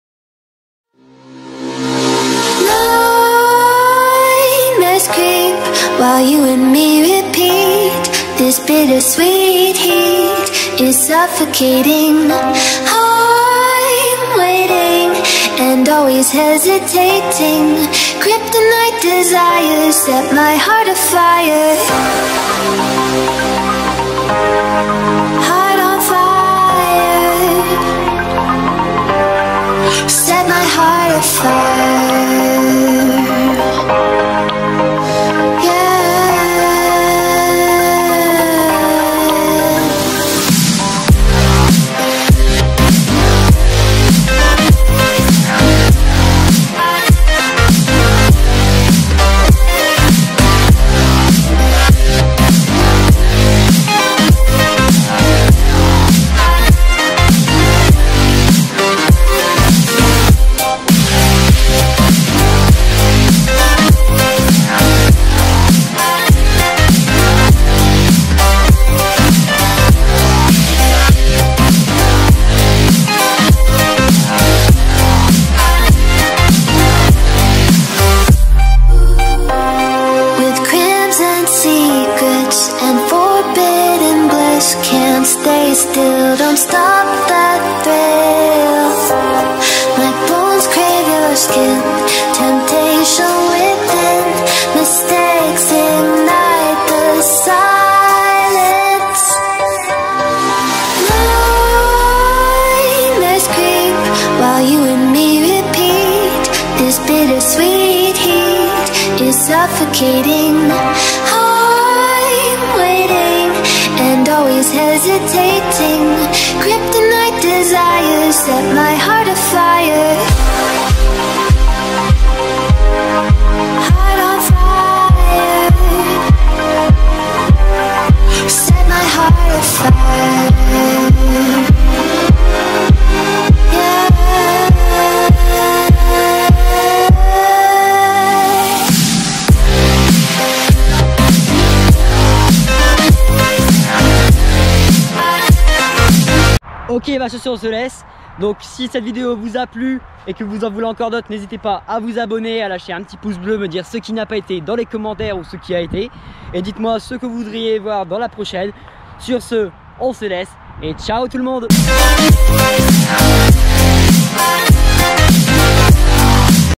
First Riding at Rieulay-Louis SZK
Salut à tous et bienvenue sur ma toute première vidéo.
Désolée si certains passages sont de mauvaises qualité car la caméra était mal réglée, sa ne se reproduira plus;)